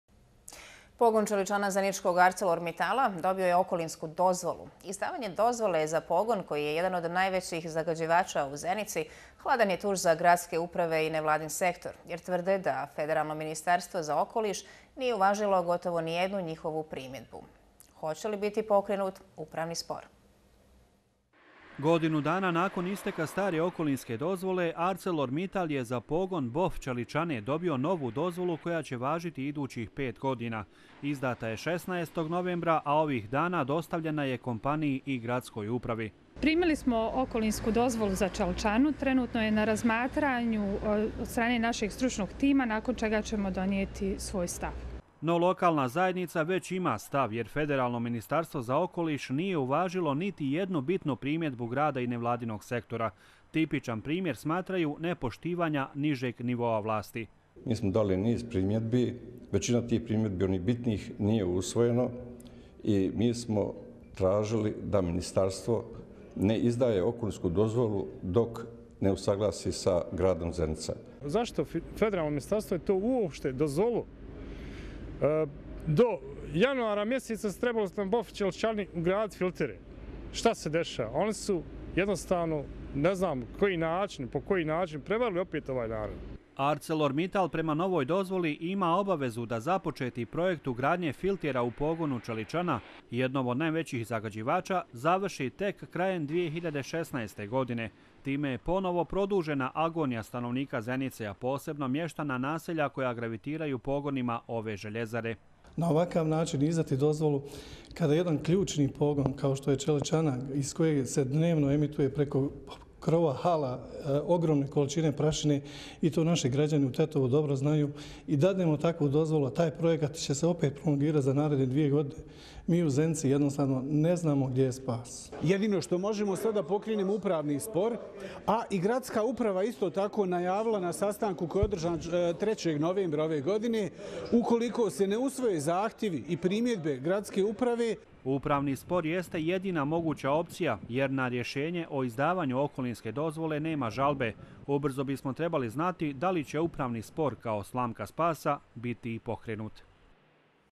Burne reakcije u Zenici nakon okolinske dozvole za Mittal
Pogon Čeličana zeničkog ArcelrMittala dobio je okolinsku dozvolu.

Izdavanje dozvole za pogon koji je jedan od najvećih zagađivača u Zenici, hladan je tuš za gradske uprave i nevladin sektor, jer tvrde da Federalno ministarstvo za okoliš nije uvažilo gotovo nijednu njihovu primjedbu. Hoće li biti pokrenut upravni spor?

Godinu dana nakon isteka stare okolinske dozvole, ArcelorMIttal je za pogon BOF čeličane dobio novu dozvolu koja će važiti idućih pet godina. Izdata je 16. novembra, a ovih dana dostavljena je kompaniji i gradskoj upravi.

No, lokalna zajednica ima stav, jer Federalno ministarstvo za okoliš nije uvažilo niti jednu bitnu primjedbu grada i nevladinog sektora. Tipičan primjer, smatraju, nepoštivanja niže nivoa vlasti.

ArcelorMittal prema novoj dozvoli ima obavezu da započeti projekt ugradnje filtera u pogonu Čeličana, jednom od najvećih zagađivača, završi tek krajem 2016. godine. Time je ponovo produžena agonija stanovnika Zenice, a posebno mještana naselja koja gravitiraju pogonima ove željezare.

Upravni spor jeste jedina moguća opcija, jer na rješenje o izdavanju okolinske dozvole nema žalbe. Ubrzo bismo trebali znati da li će upravni spor, kao slamka spasa, biti i pokrenut.